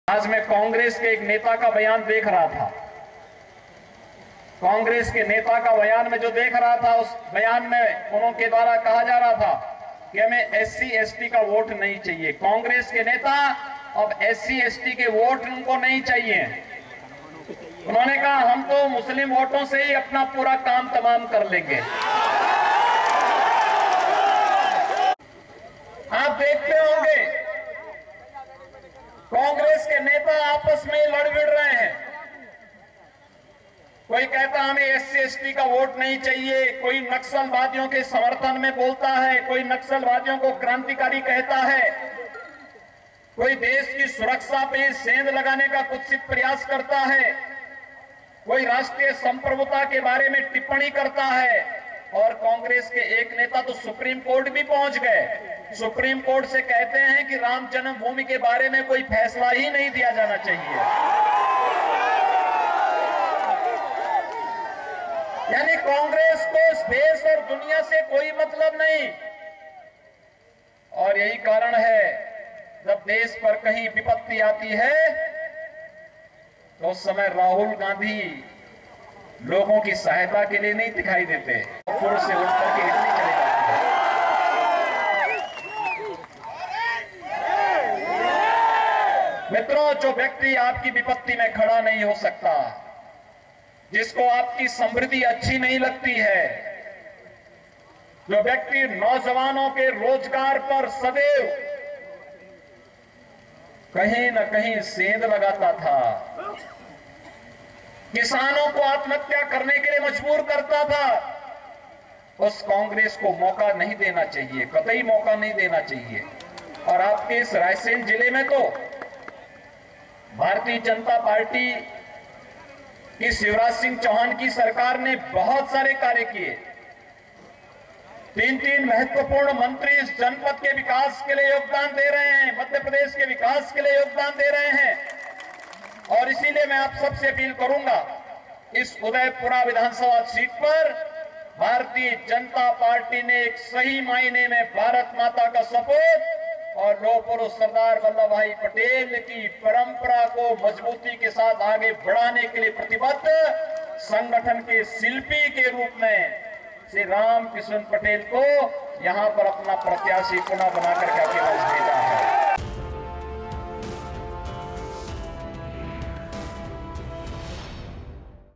MP-चुनाव: 'कांग्रेस को एससी-एसटी का वोट नहीं चाहिए'| MP Tak
एमपी कांग्रेस अध्यक्ष कमलनाथ के वीडियो वायरल  पर यूपी सीएम योगी आदित्यनाथ ने रायसेन आयोजित चुनावी रैली में जमकर हमला बोला.